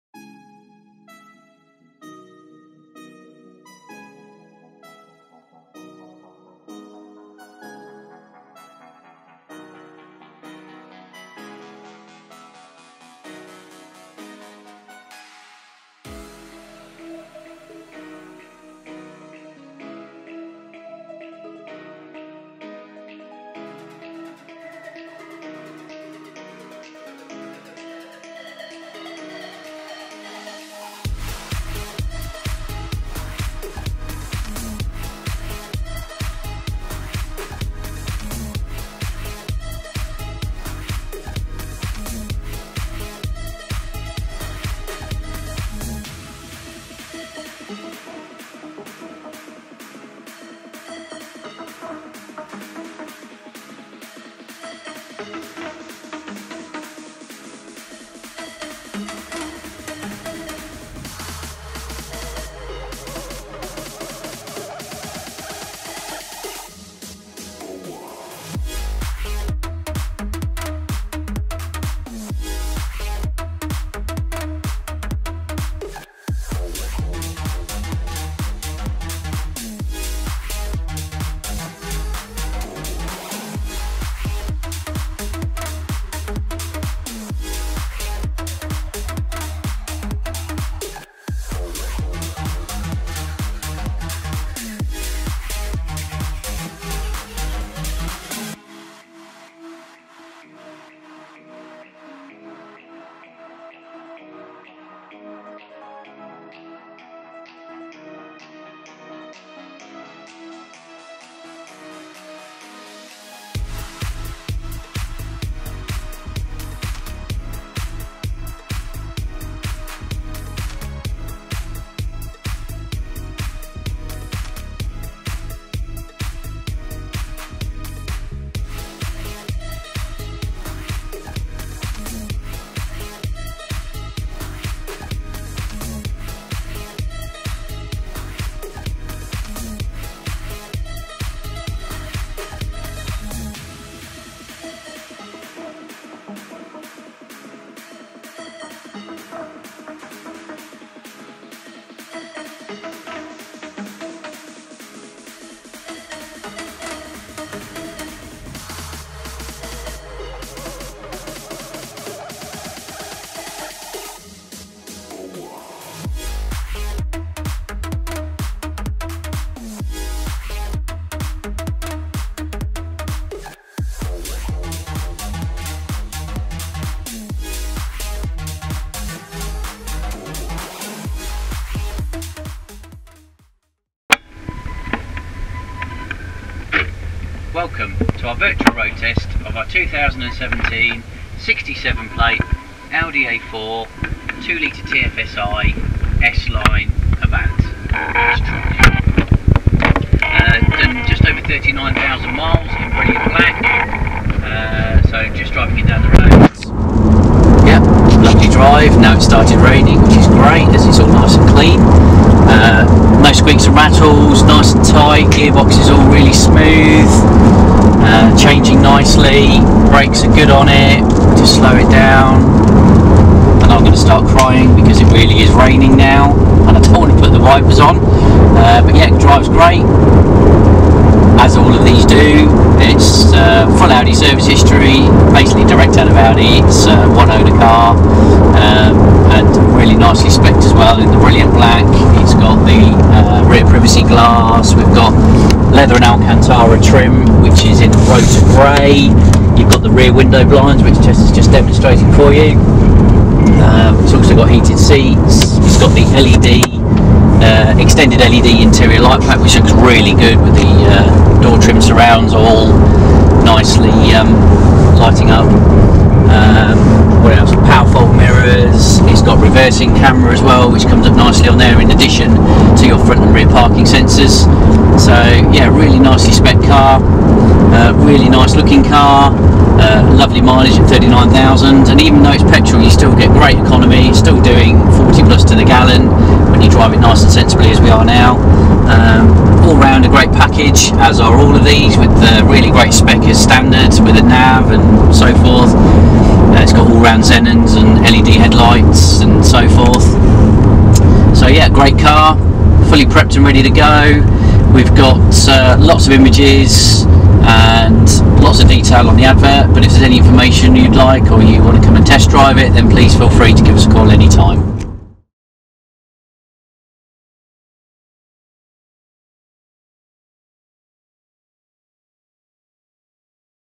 2017:67 Audi A4 2.0 TFSI S-Line Avant S-Tronic
A Fantastic  Example, 1 Company Owner, With Full Audi Service History, Huge Spec!  Leather/Alcantara-Rotor Grey Trim, Heated front seats, Rear View Camera, Extended LED Interior Light Pack, 3 Spoke Flat Bottom Steering Wheel, Rear Privacy Glass, Side Window Blinds, Piano Finish Black, Dynamic Suspension, Power Fold Mirrors, LED headlight / high beam assist, Technology package, Apple Car Play. Full Dealer Facilities.